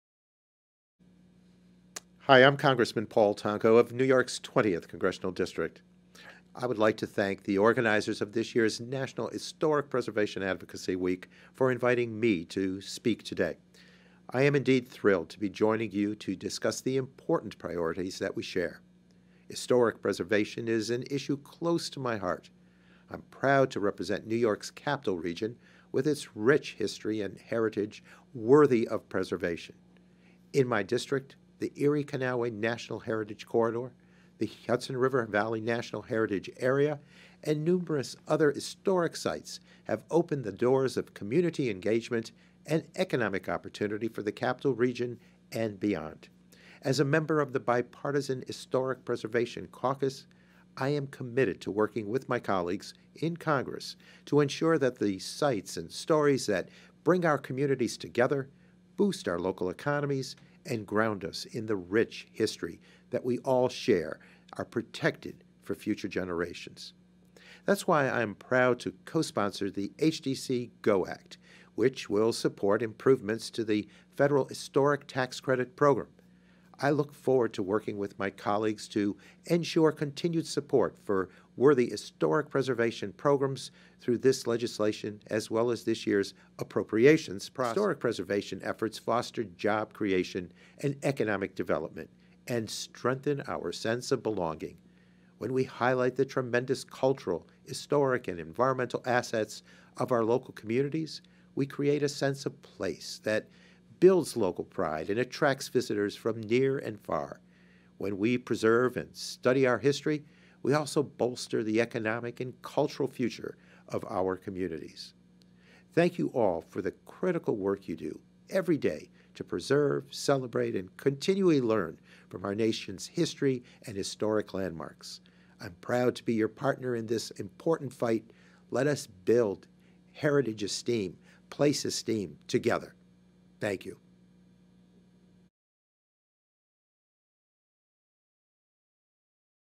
Advocacy Week 2024- Video Message From Rep. Paul Tonko
Video message from Congressman Paul Tonko (D-NY) featured during Historic Preservation Advocacy Week 2024